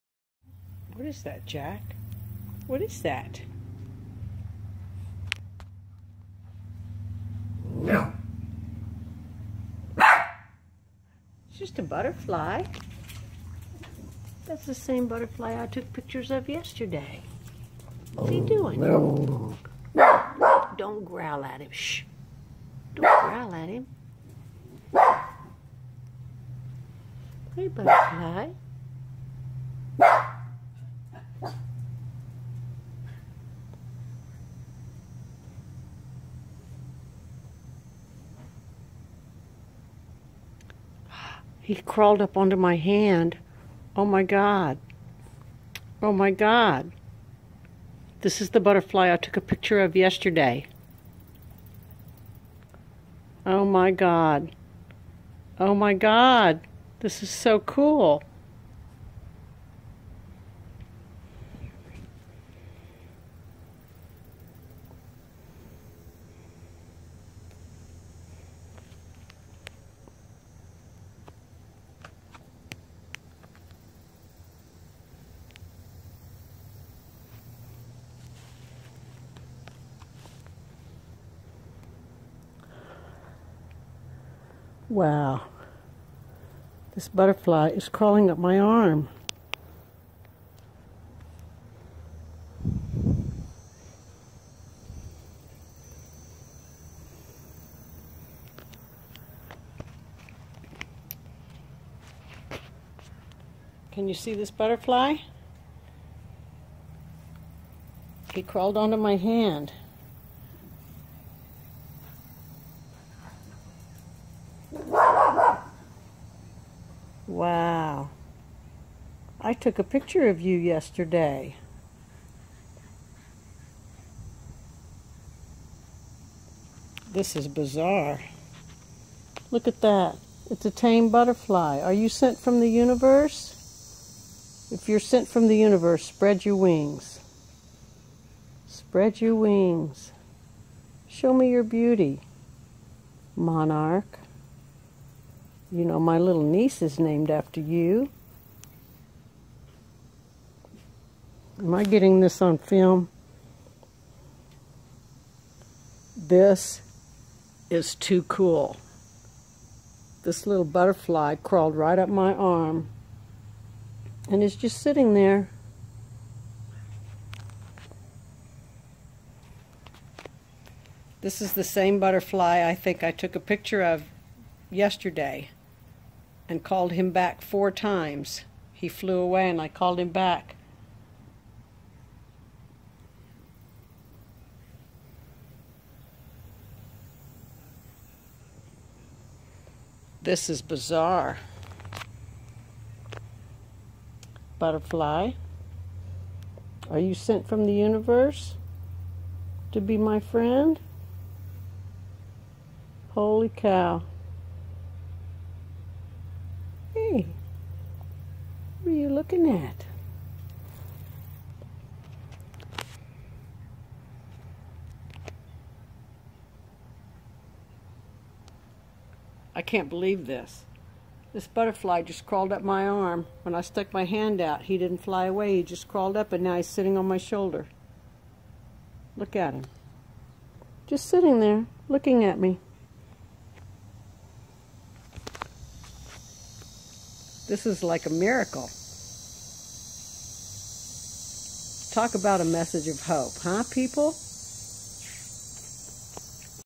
Butterfly Whisperer
What happens when a butterfly decides he likes you.